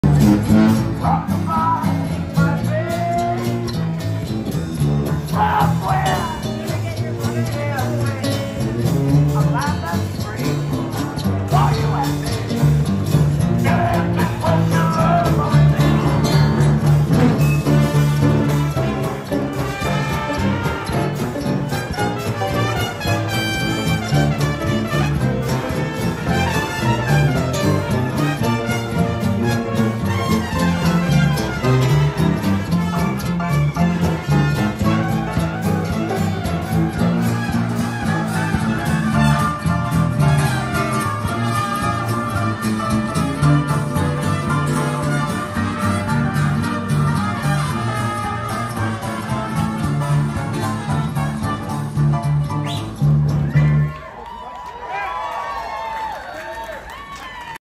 Live music at the 2024 French Quarter Festival in New Orleans
A band performs on Royal Street in the French Quarter during the annual French Quarter Festival in New Orleans.